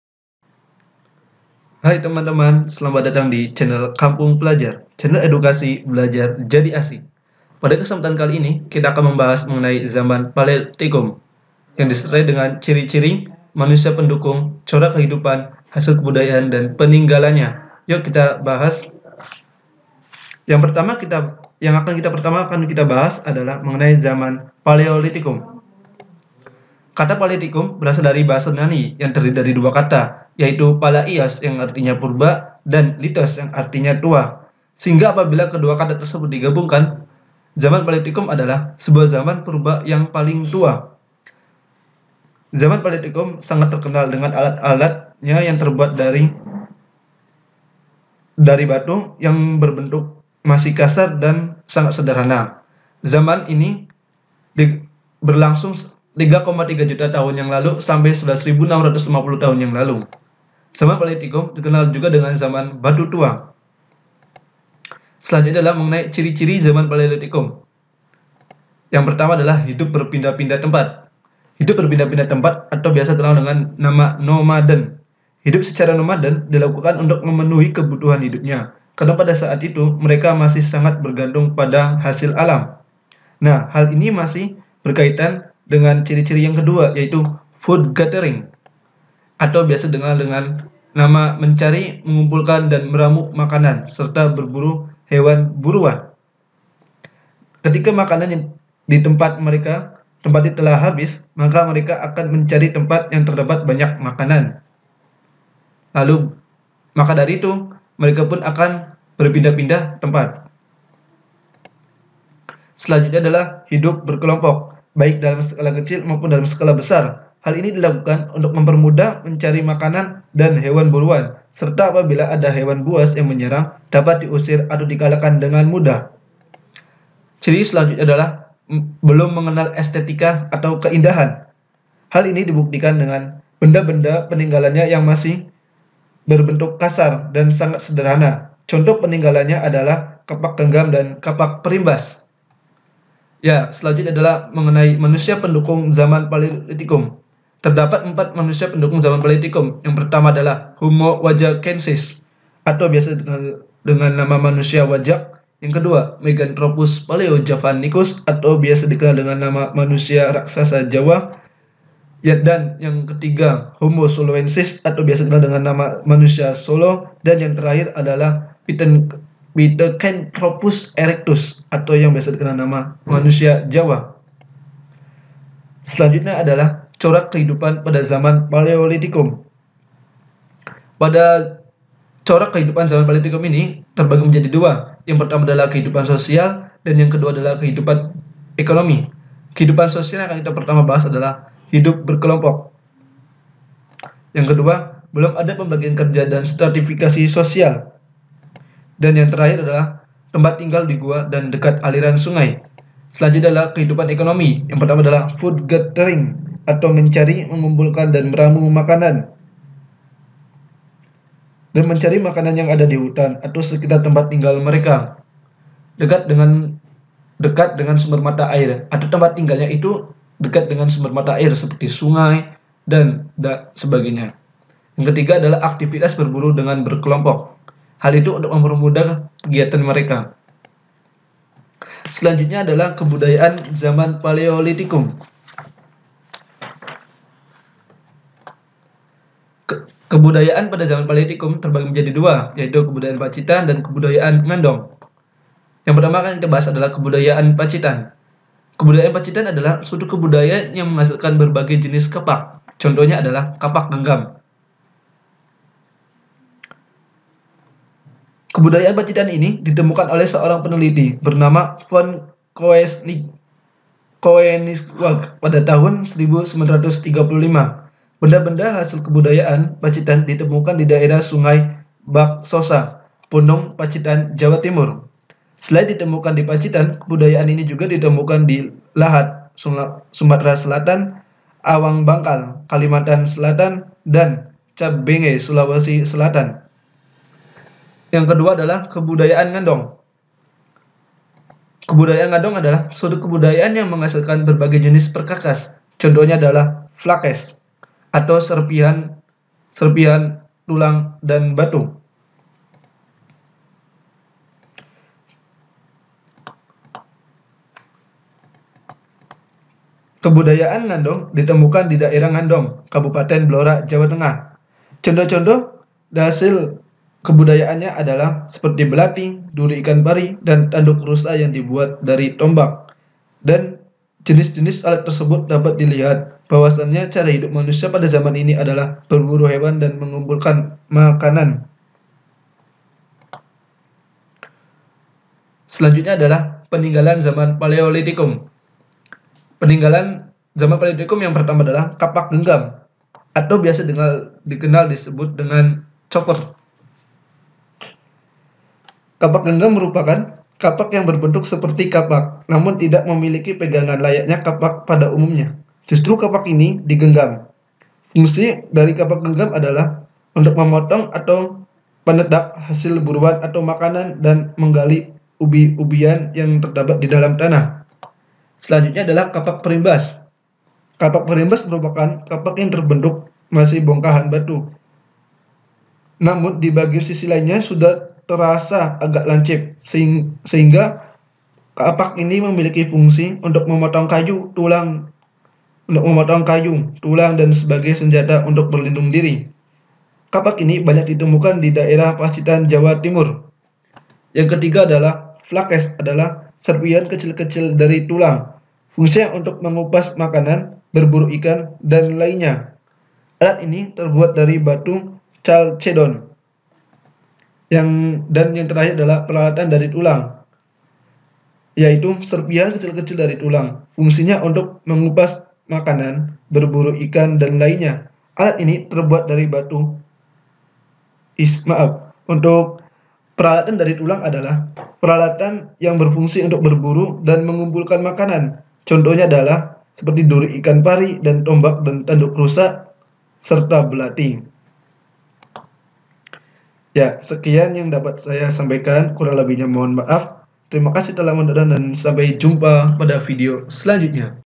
SEJARAH - Kehidupan Zaman Paleolitikum Lengkap
Zaman paleolitikum merupakan zaman batu tua yang pertama dalam pembagian zaman batu. Dalam materi ini akan membahas lengkap mengenai kehidupan zaman paleolitikum seperti ciri - ciri, manusia pendukung, kebudayaan, peninggalan dan masih banyak lagi lainnya.

Bagi sobat yang ingin membaca versi artikel, silahkan klik link dibawah ini.

Hubungi saya melalui berikut ini.